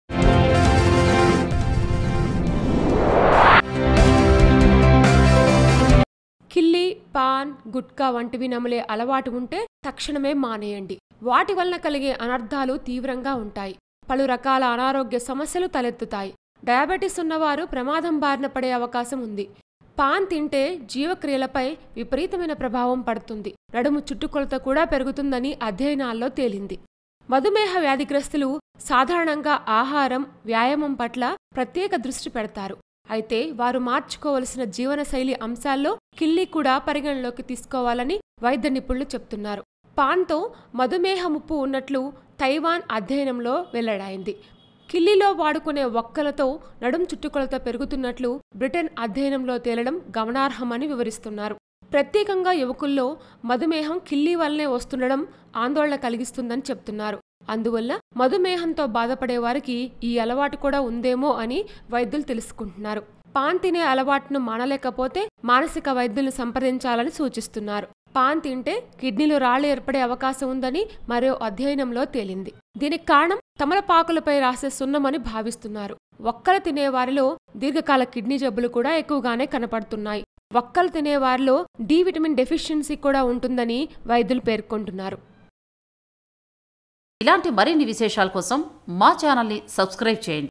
Disadvantages Of Pan Taking | కిళ్లీ,పాన్,గుట్కా వంటివి నమిలే..కిడ్నీ,మధుమేహం..!
Watch :Disadvantages Of Pan Taking | కిళ్లీ,పాన్,గుట్కా వంటివి నమిలే..కిడ్నీ,మధుమేహం..!

Aarogya Sutra